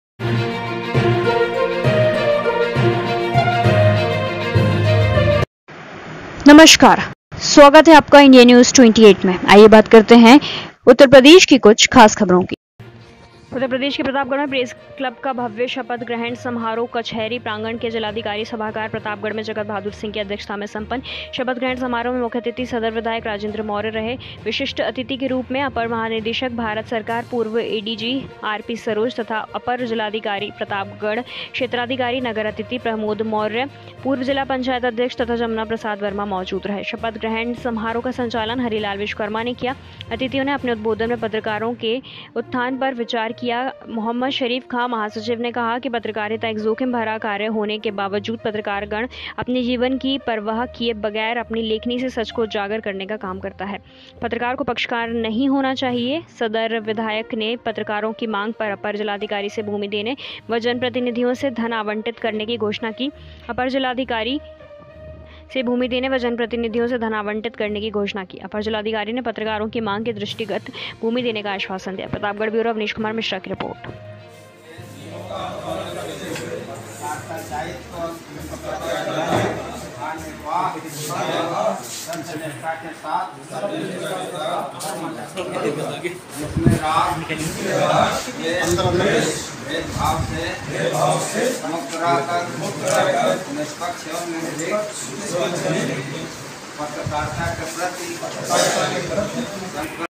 उत्तर प्रदेश के प्रतापगढ़ में प्रेस क्लब का भव्य शपथ ग्रहण समारोह
Official